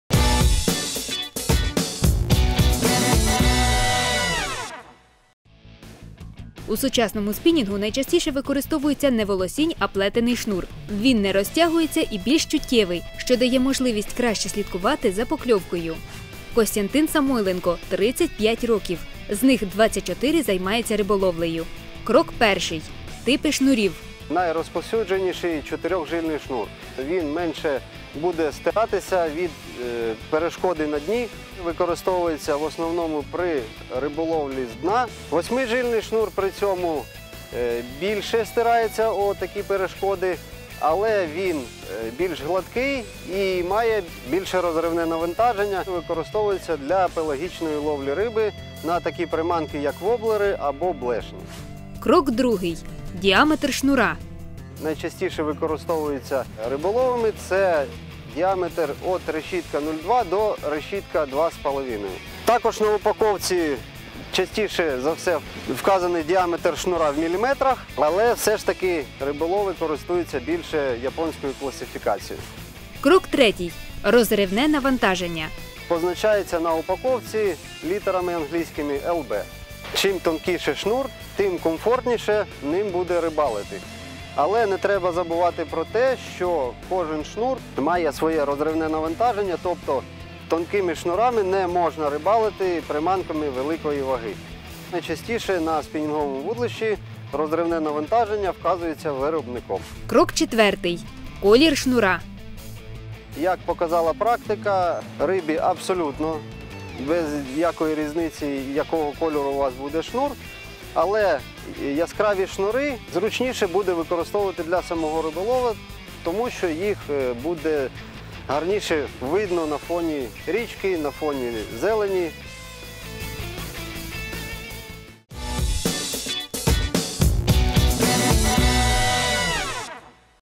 Поради рибалкам: вибираємо шнури для спінінга | Ранок на Суспільному
У сучасному спінінгу найчастіше використовується не волосінь, а плетений шнур. Він не розтягується і більш чутливий, тому дає можливість краще слідкувати за покльовкою. Про типи, діаметр та розривне навантаження розповідає рибалка з 24-річним досвідом, Костянтин Самойленко.